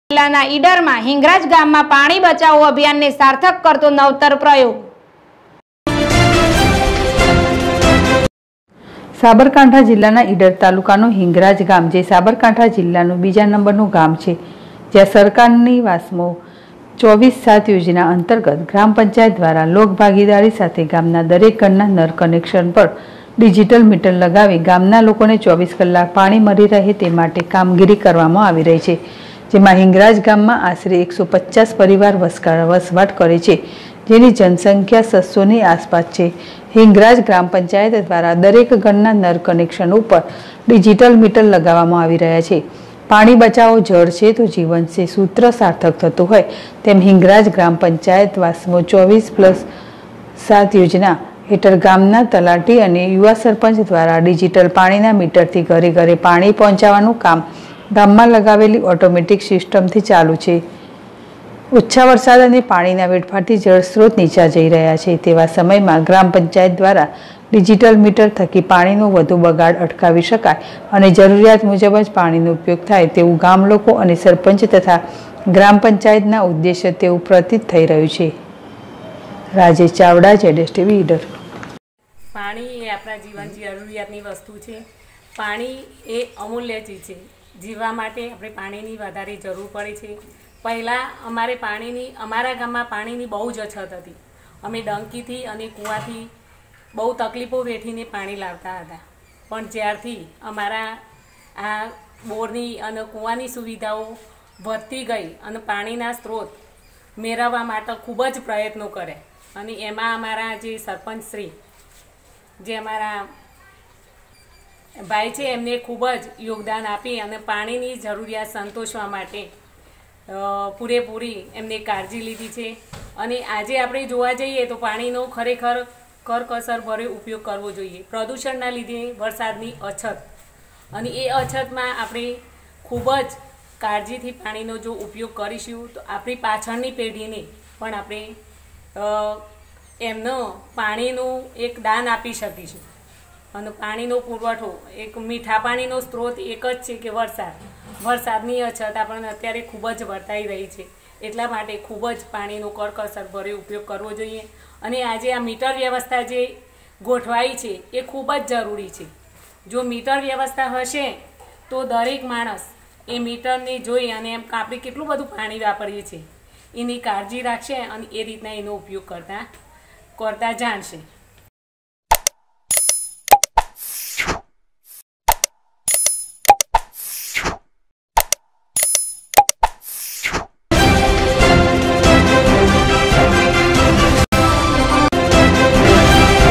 ઇડરના હિંગળાજ ગામમા ' પાણી બચાવો ' અભિયાનને સાર્થક કરતો નવતર પ્રયોગ
ઇડર સાબરકાંઠા જિલ્લાના ઇડરના હિંગળાજ ગામમા ' પાણી બચાવો ' અભિયાનને સાર્થક કરતો નવતર પ્રયોગ